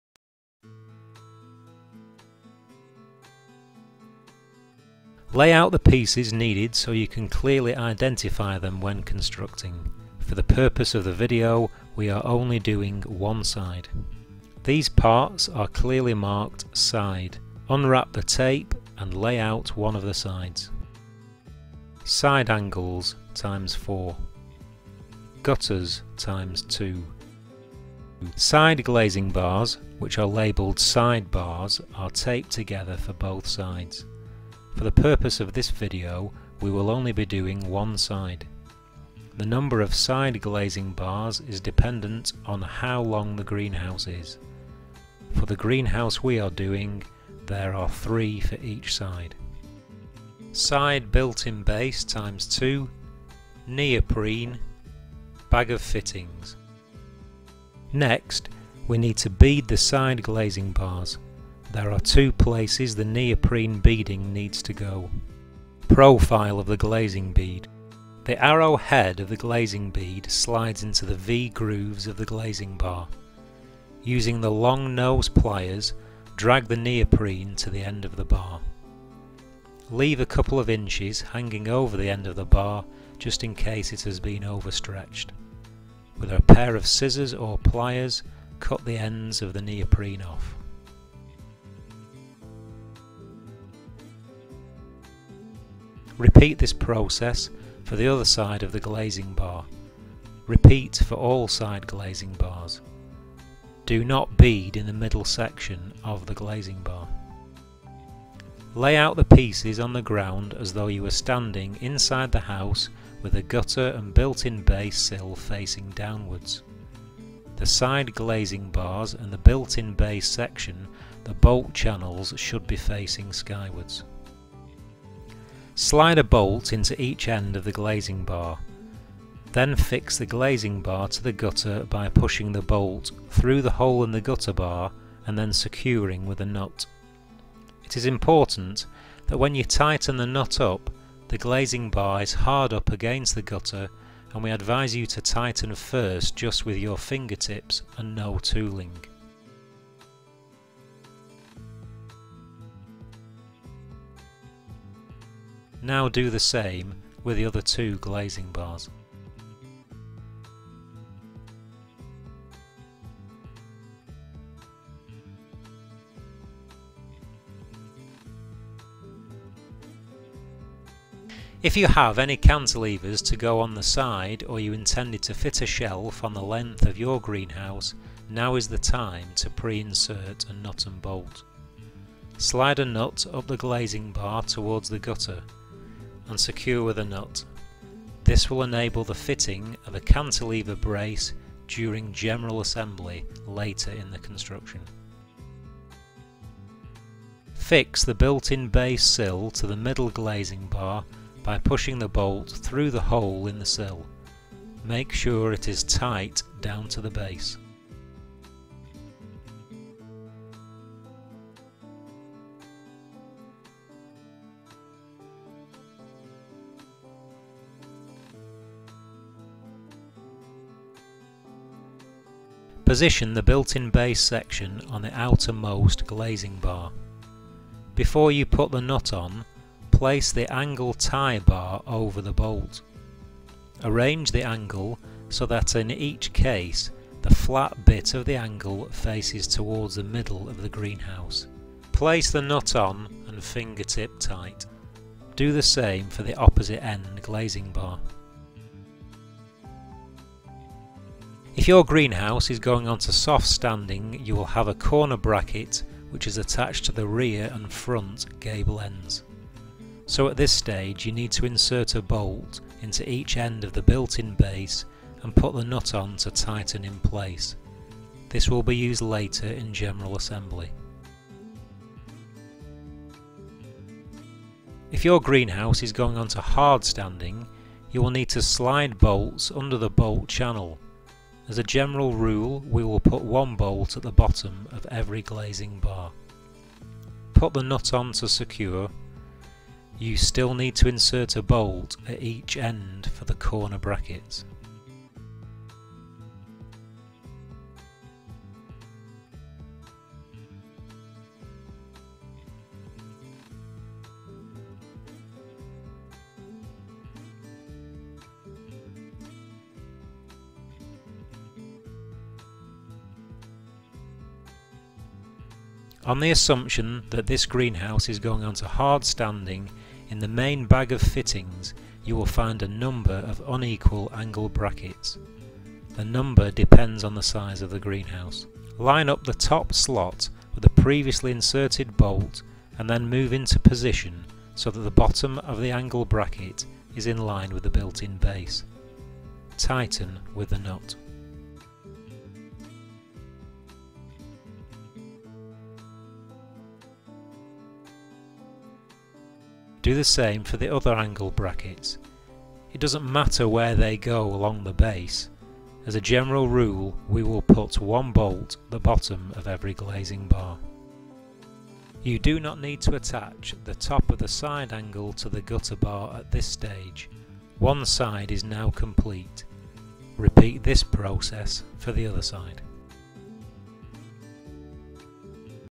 Elite Self-Build Video - Side Frame Assembly
All the information you need on how to make the side frame assembly of your Elite Greenhouse is here in this video.